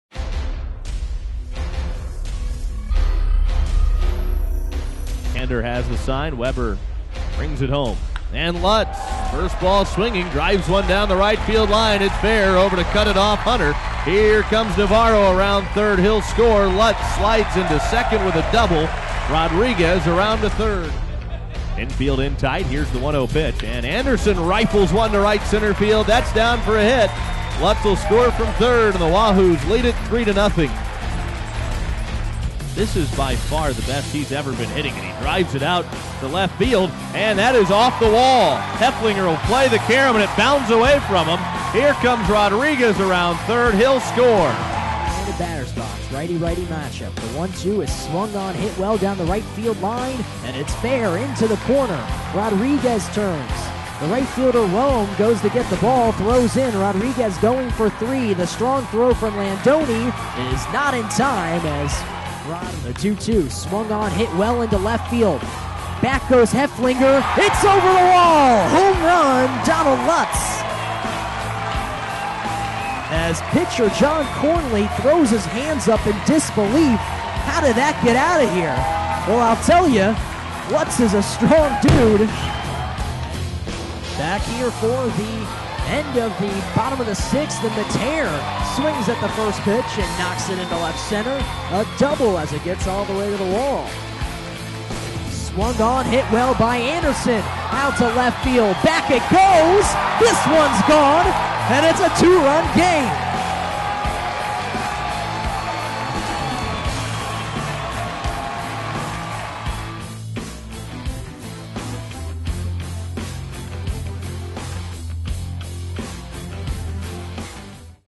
Blue Wahoos Highlights 4-24-14
sellout crowd turned out at Pensacola Bayfront Stadium Thursday night as the Blue Wahoos honored two-time Master's champ Bubba Watson. The celebration was short lived as the Blue Wahoos dropped the first game of the series 11-9 to the visiting Mississippi Braves. The Wahoos (8-13) had a season-high five errors, which led to six unearned runs for the Braves (8-13).